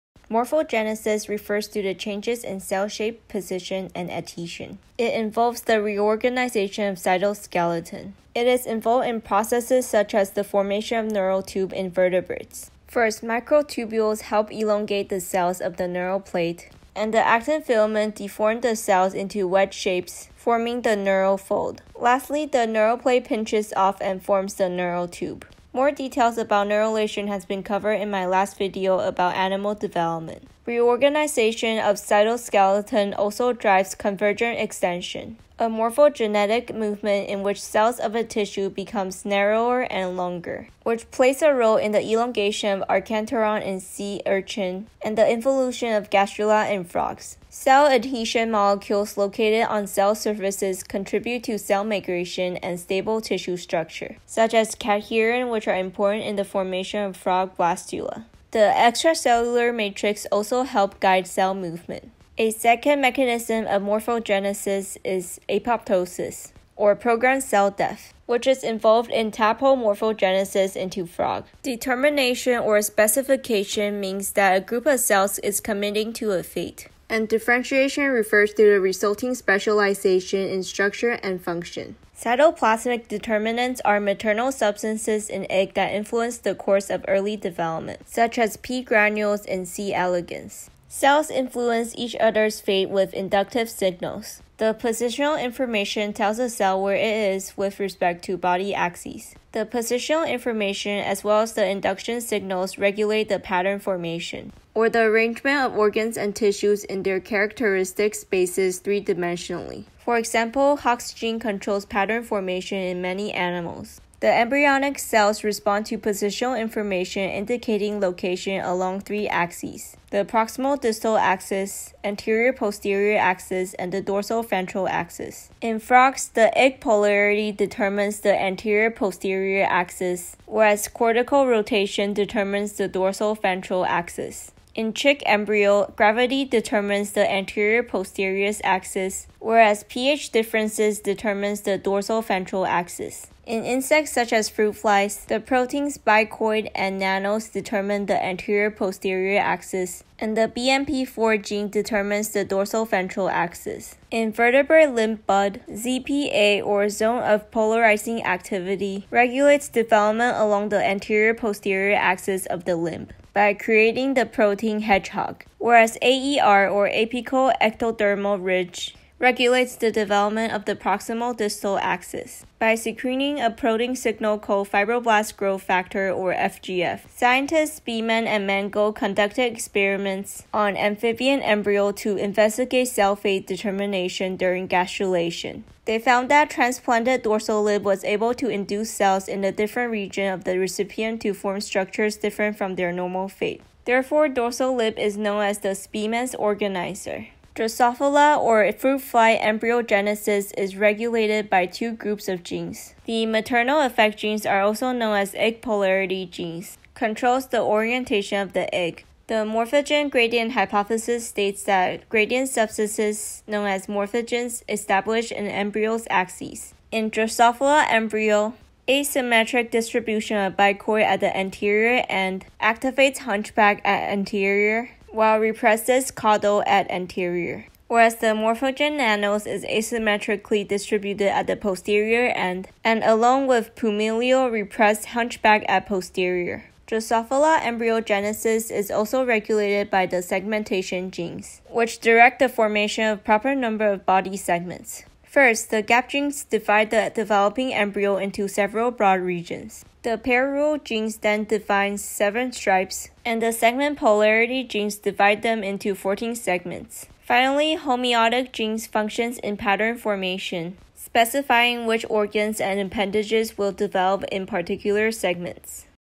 Cell Fate Specification | Animal Physiology 13 | Biology | PP Notes | Campbell Biology 8E Ch. 47
A summary review video about cell fate specification.

Timestamps:
0:00 Morphogenesis
1:11 Key Terms (determination, differentiation, cytoplasmic determinants, induction signals, positional information, and pattern formation)
1:53 Axis Formation (in frog, chick, insects, limb bud, )
3:17 Drosophila Embryogenesis (maternal effect genes, segmentation genes)